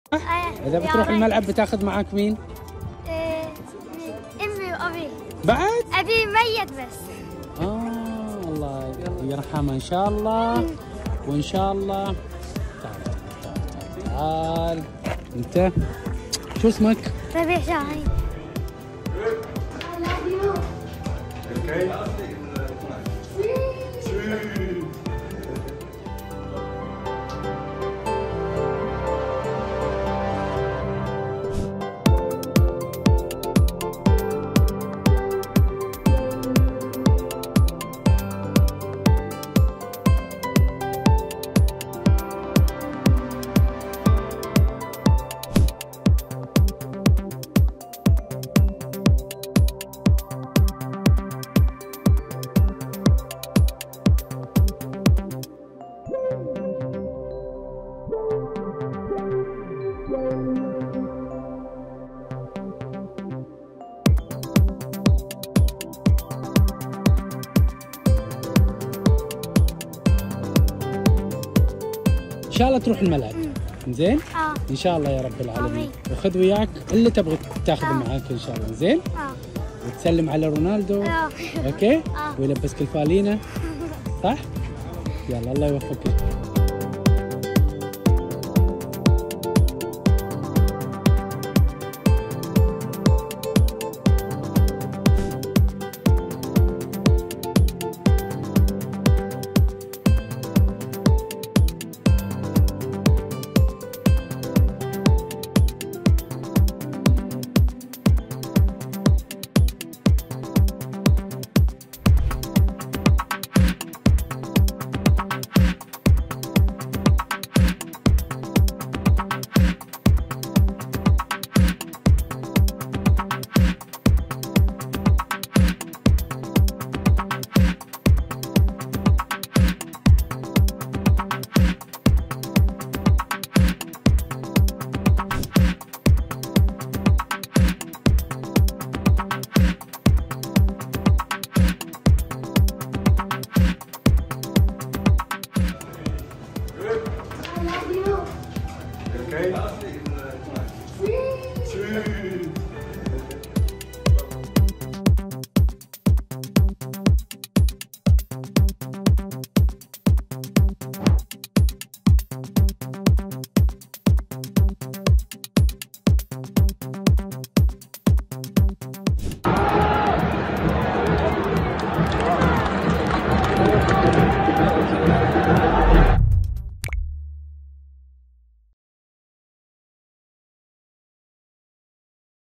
"أبي ميت بس".. الطفل السوري اليتيم يحقق حلمه ويقابل الأسطورة كريستيانو رونالدو
حقق الطفل السوري ربيع شاهين حلمه بعدما تمنى رؤية الأسطورة البرتغالية كريستيانو رونالدو.